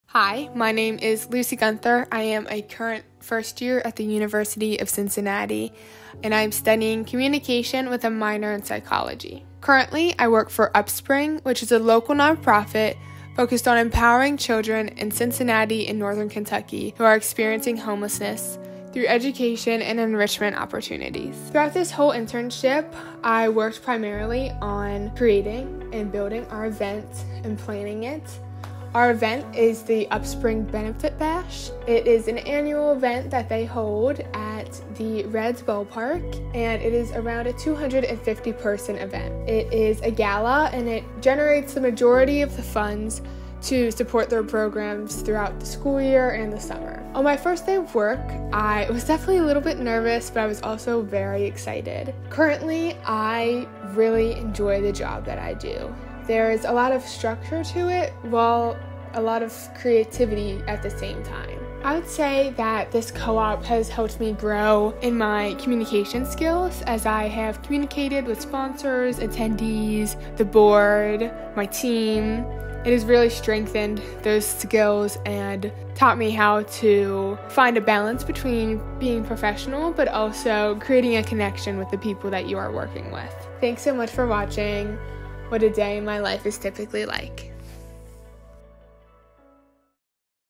A Day in the Life of a UC Service-Learning Co-op Student
University of Cincinnati  student Lucy Gunther takes you behind the scenes of her co-op and talks about her service-learning co-op experience.